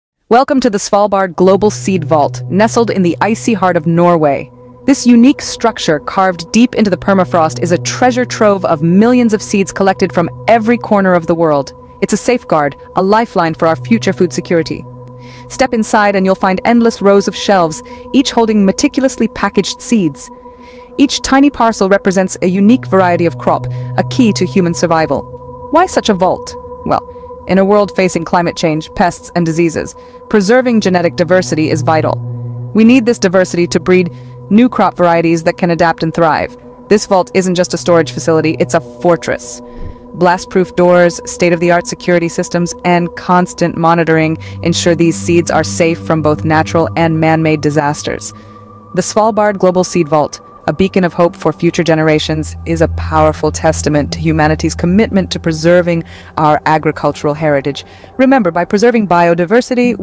Exploring the Svalbard Global Seed Vault
Welcome to our latest video where we delve into the incredible world of the Svalbard Global Seed Vault located in Norway! Join us as we uncover the importance of this vital facility in ensuring the preservation of our world's plant biodiversity and securing our future food supply.